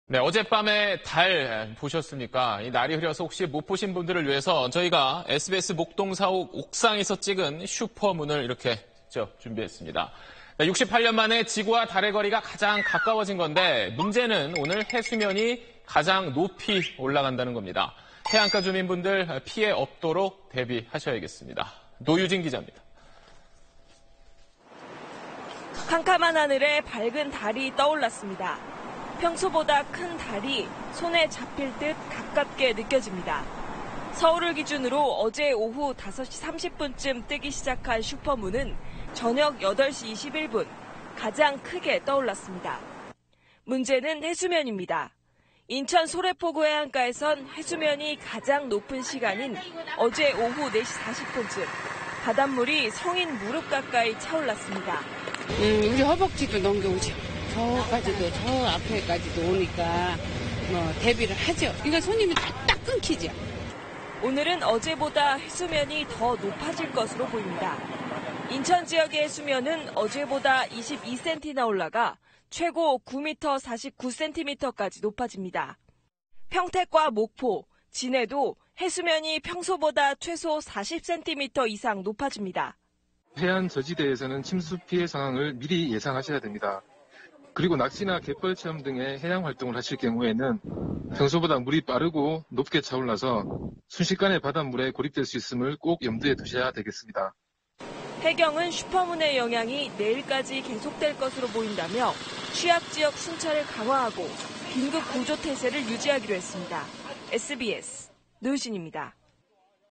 68년 만의 슈퍼문…해수면 오늘 최고조 '비상' / SBS
어젯(14일)밤에 혹시 달 보셨습니까? 날이 흐려서 혹시 못 보신 분들을 위해서 저희가 SBS 목동 사옥 옥상에서 찍은 슈퍼문을 이렇게 직접 준비했습니다. 68년 만에 지구와 달의 거리가 가장 가까워진 건데, 문제는 오늘 해수면이 가장 높이 올라간다는 겁니다. 해안가 주민분들 피해 없도록 대비하셔야겠습니다.

노유진 기자입니다.